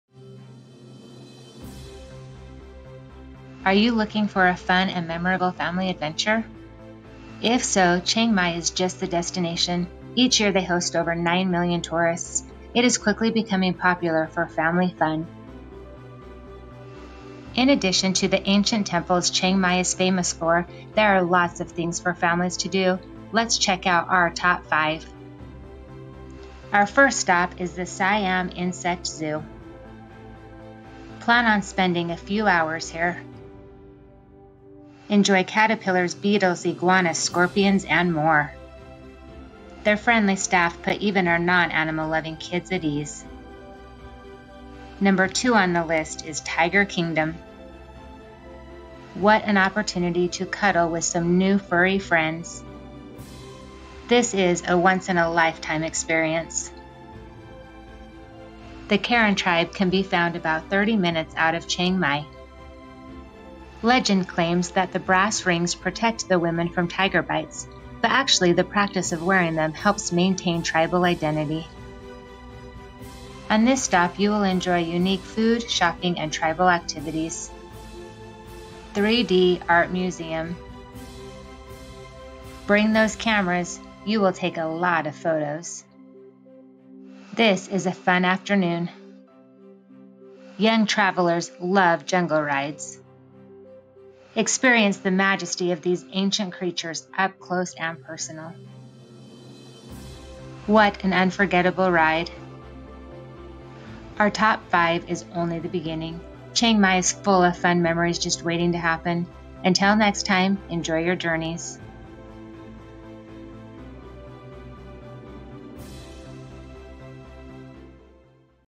Chiang Mai, Thailand with Kids
Our Family's top 5 things to do with kids in Chiang Mai, Thailand